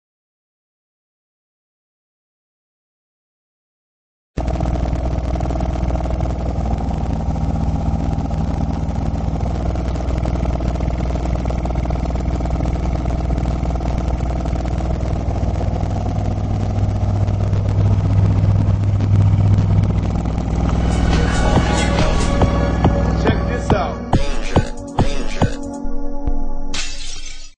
12vdb.com - car audio database: Adire Audio Brahma10.5
BHTX`s 10 Brahma MKII in a Nolte Audio Snail Shell powered by a Dayton HPSA1000 plate amp. He`s playing a 20Hz sine wave.